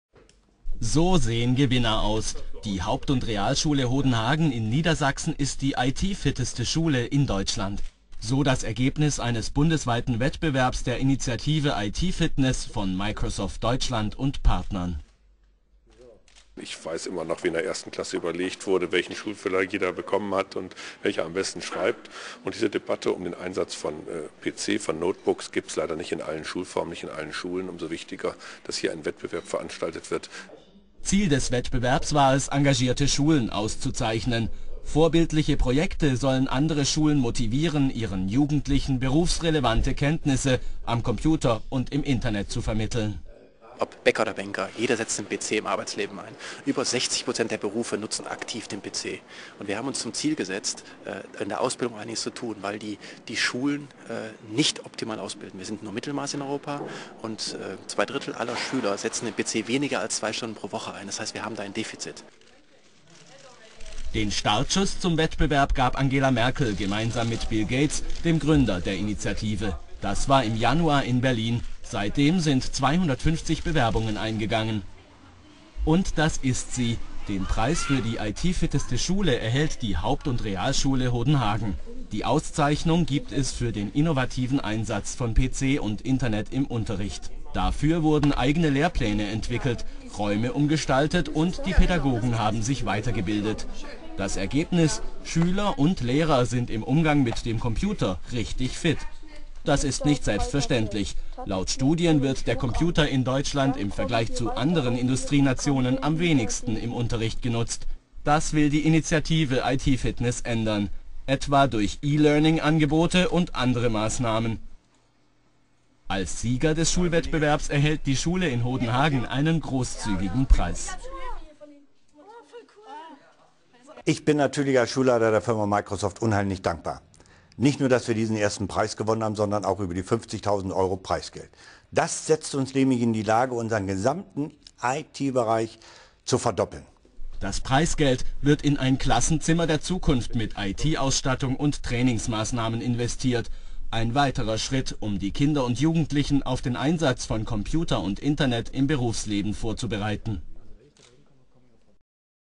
Initiative IT-Fitness prämiert beste Computer-Schule
Christian Wulff und die Initiative IT-Fitness prämieren beste "Computer-Schule" Deutschlands. Niedersächsische Schule (Hodenhagen) gewinnt bundesweiten Wettbewerb „IT-Fitness macht Schule".In Deutschlands Schulen besteht Handlungsbedarf - zwei Drittel der Schüler arbeitet im Unterricht kaum mit dem Computer. Um Schulen, Lehrer und Schüler zu motivieren, PC und Internet gezielt einzusetzen, haben Bundeskanzlerin Angela Merkel und Microsoft-Gründer Bill Gates im Januar 2008 den bundesweiten Wettbewerb „IT-Fitness macht Schule" gestartet.